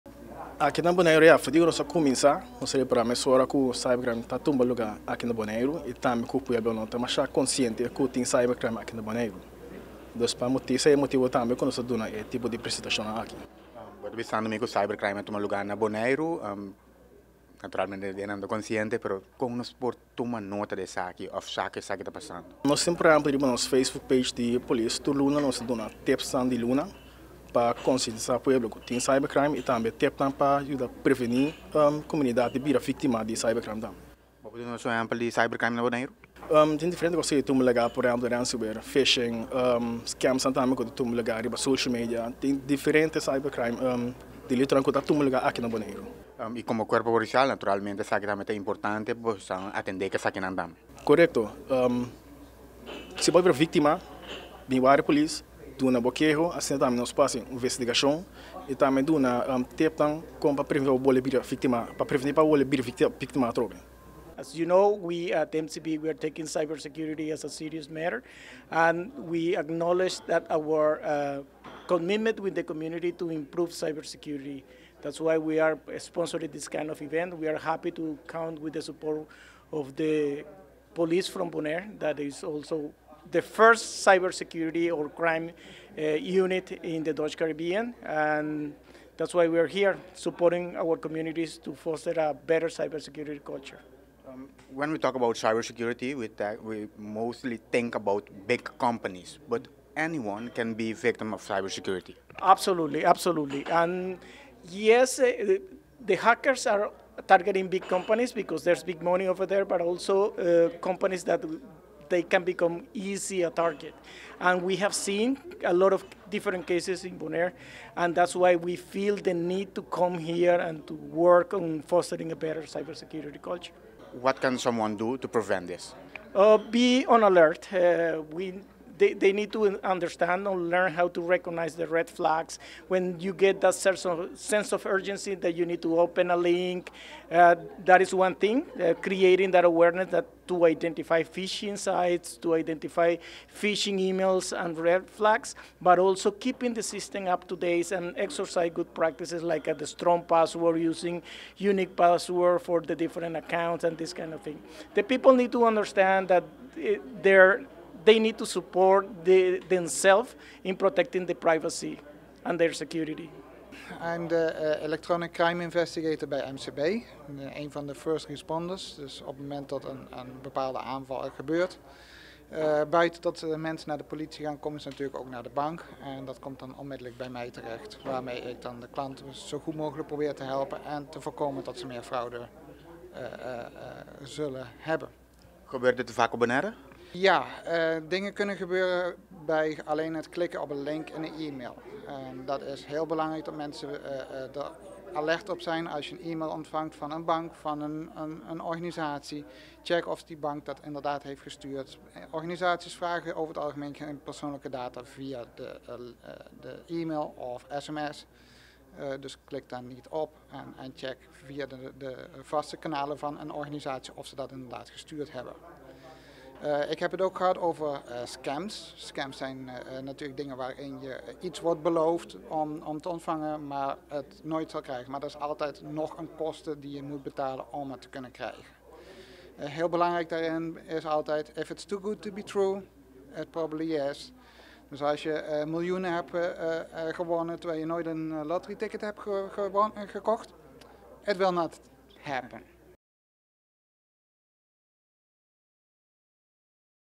1| MCB Seminario di Krimen Sibernetiko.
MCB resientemente a organisa un Seminario di Krimen Sibernetiko pa diferente partnernan di interes, den e seminario aki a trata diferente aspekto di krimen sibernetiko, Notisia Nos TV tabata presente i a kombersa ku diferente di e oradornan di e seminario.